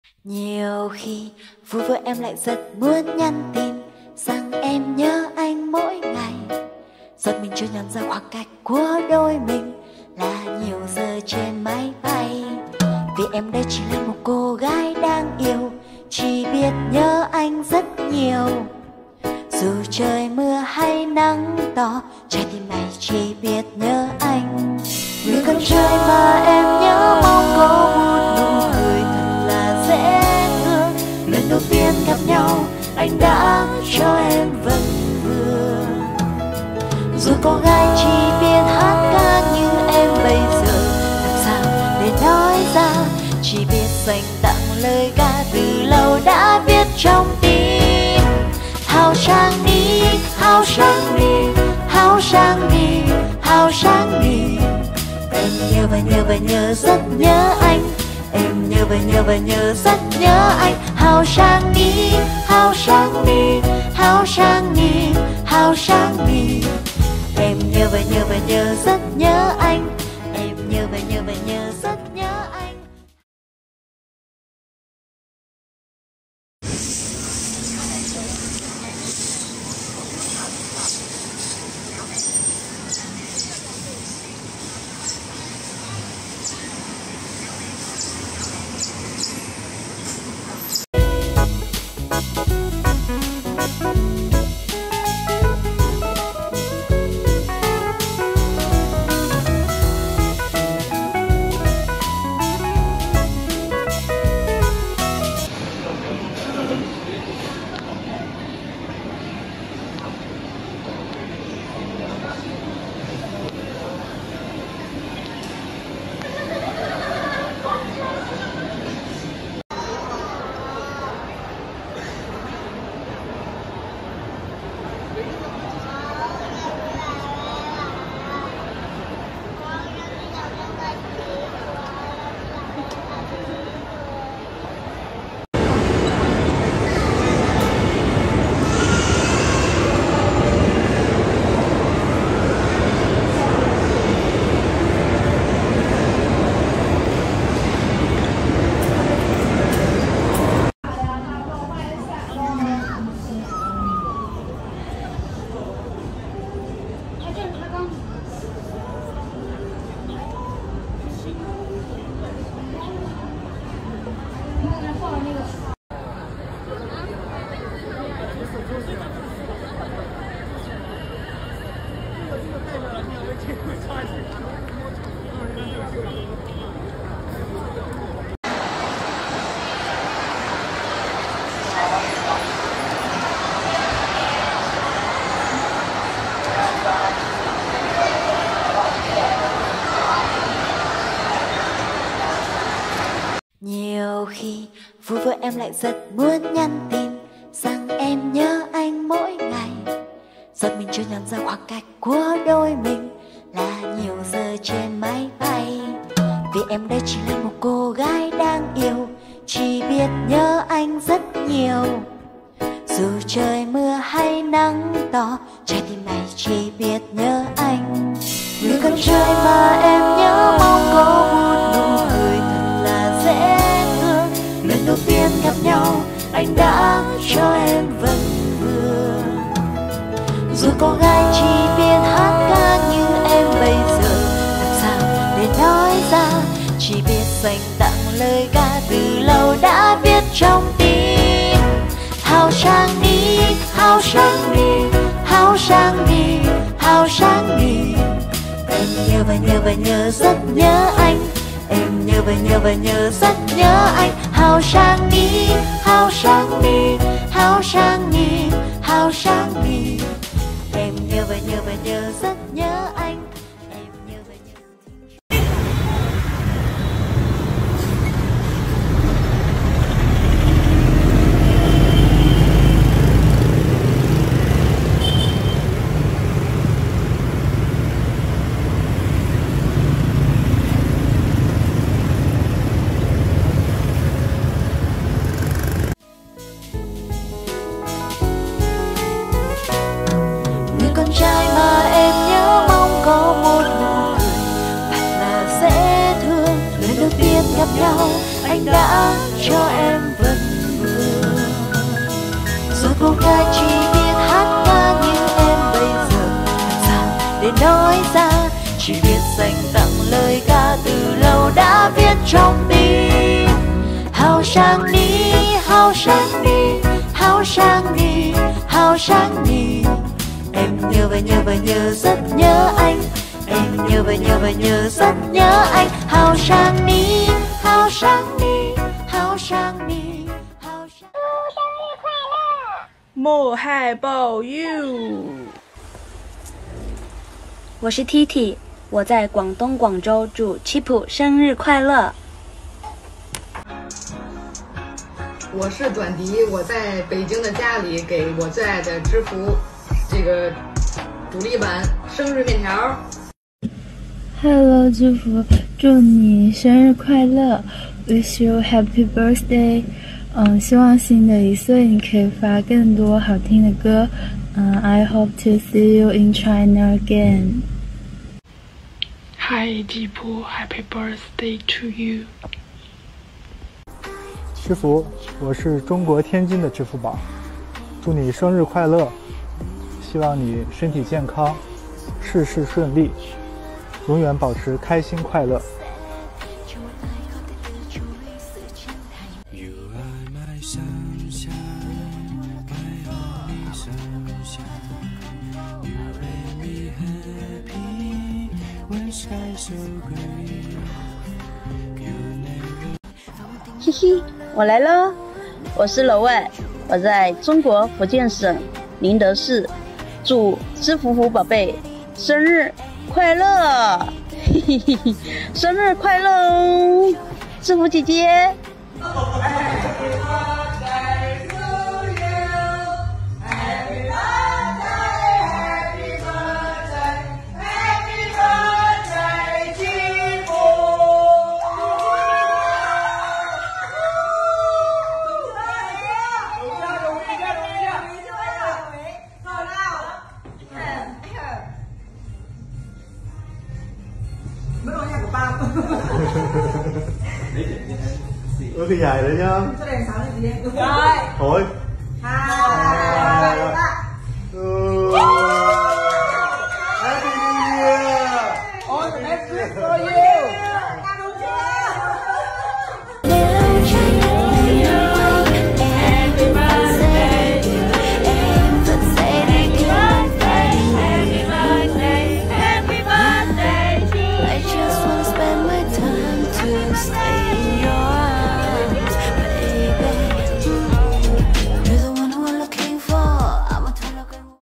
6.14 Mango TV chúc sinh nhật, rộ tin Chi Pu tham gia khách mời show Nhà hàng Trung Hoa
Mango TV chúc sinh nhật, rộ tin Chi Pu tham gia khách mời show Nhà hàng Trung Hoa, fan Trung - Việt chúc sinh nhật ChiPu